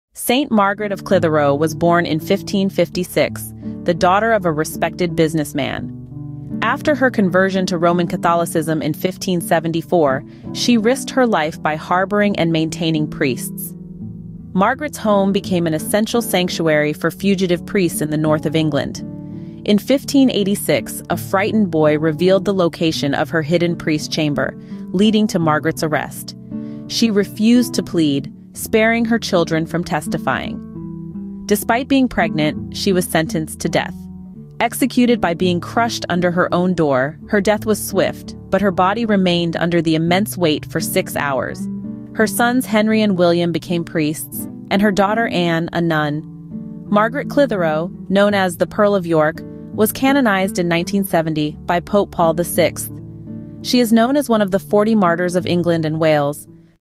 Saint Margaret Clitherow - A Martyr's Courage | Catholic Saints Series
Step into the rich tapestry of Catholic history as we delve into the life of Saint Margaret Clitherow, a remarkable figure of unwavering faith and steadfast devotion. Join us in this short video as we explore the inspiring story of this courageous martyr and patroness of businesswomen, converts, and martyrs.

Through vivid storytelling and historical insights, we uncover Margaret's extraordinary journey from her conversion to Catholicism during a time of religious persecution in Elizabethan England to her ultimate sacrifice for her faith. Learn about her clandestine efforts to harbor priests and promote Catholicism, defying oppressive laws at great personal risk.

Witness the unwavering strength of Margaret's faith as she faced imprisonment, torture, and ultimately martyrdom for her refusal to renounce her beliefs. Discover how her legacy continues to inspire countless faithful around the world, encouraging us to stand firm in our convictions despite adversity.

Join us on this spiritual journey as we honor the memory of Saint Margaret Clitherow and reflect on her enduring legacy of faith, courage, and martyrdom. Let her story ignite a flame of devotion within our hearts and inspire us to live boldly for our Catholic faith.